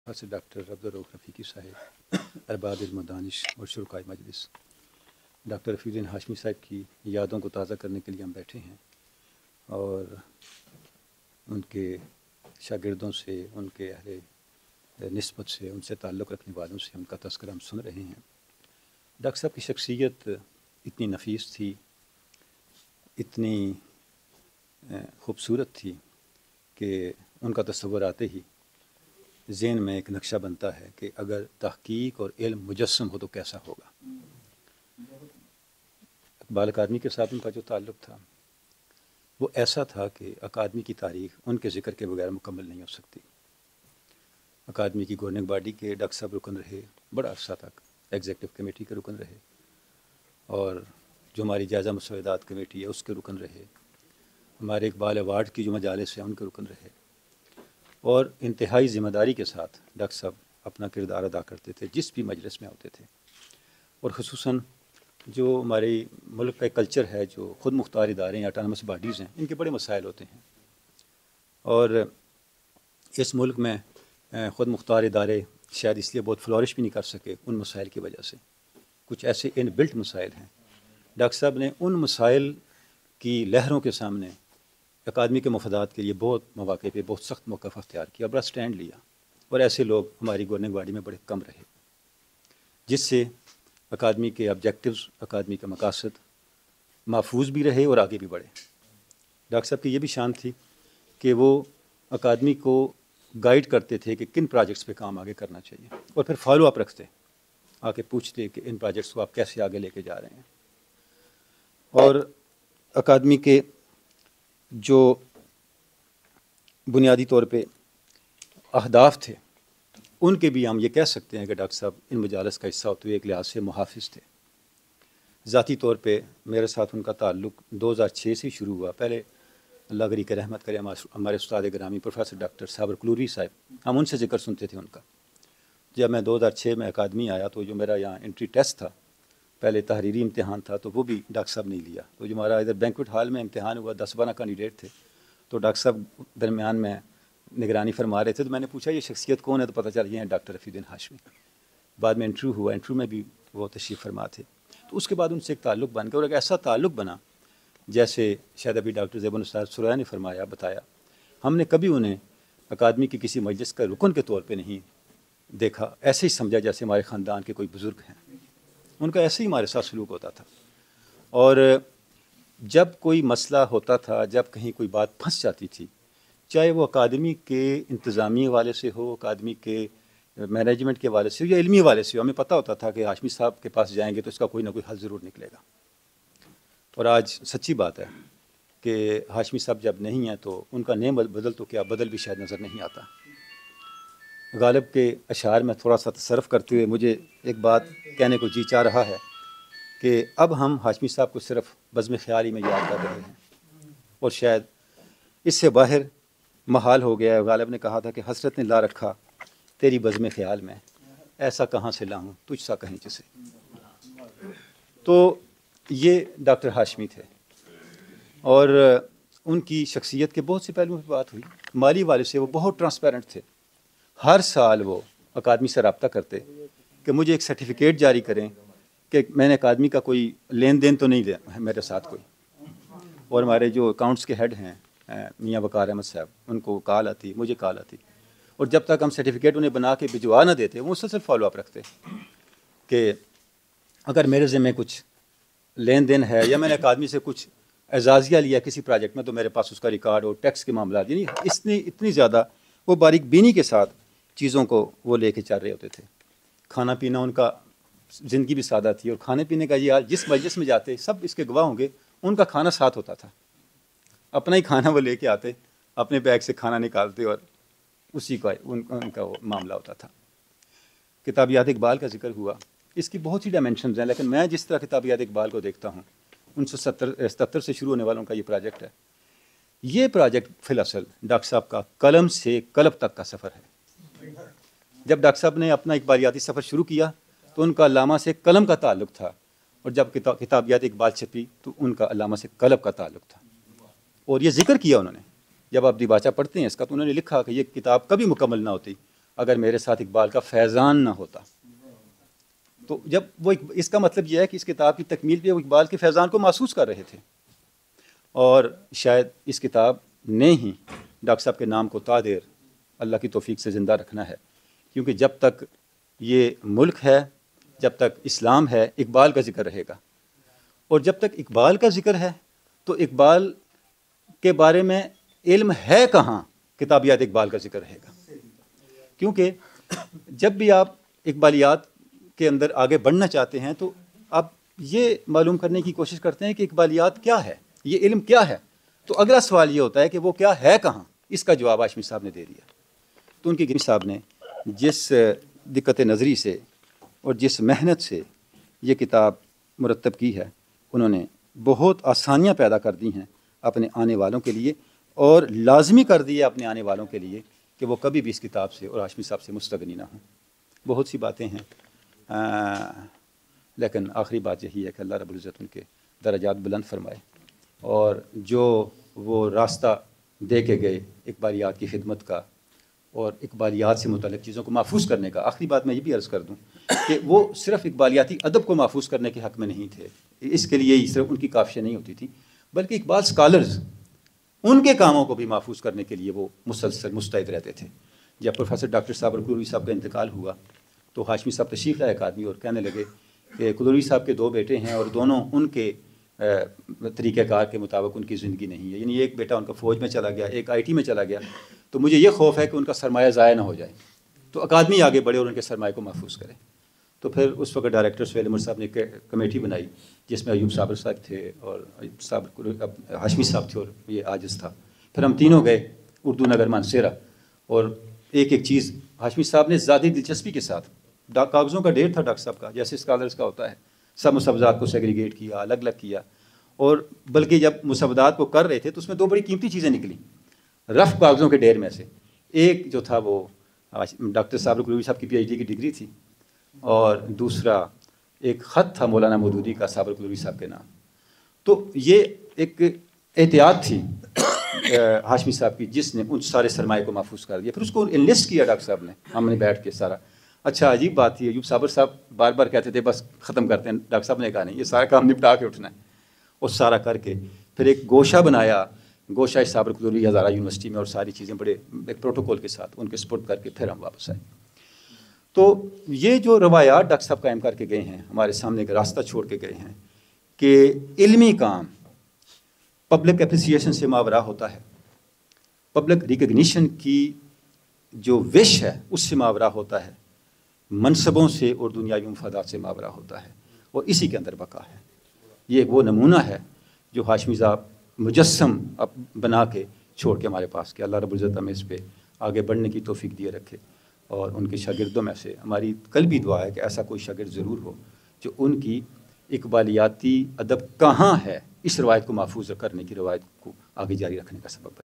Iqbal Academy: Condolence Reference: Prof. Dr. Rafiuddin Hashmi (Late) : Dr. Tahir Hameed Tanoli
Iqbal Academy Pakistan has conducted a Condolence Reference to pay tribute to Late Prof. Dr. Rafiuddin Hashmi, renowned Teacher, Intellect, Writer, Researcher,  Iqbal Scholar on 31th January 2024 in Committee Room No. 2, Aiwan e Iqbal Complex, Lahore. Prof. Dr. Rafiuddin Hashmi's Son, Daughter, Brother, friends, colleagues expressed their views about his life and research work.

This is a part of outreach activities of Iqbal Academy Pakistan under the supervision of Director Iqbal Academy Pakistan.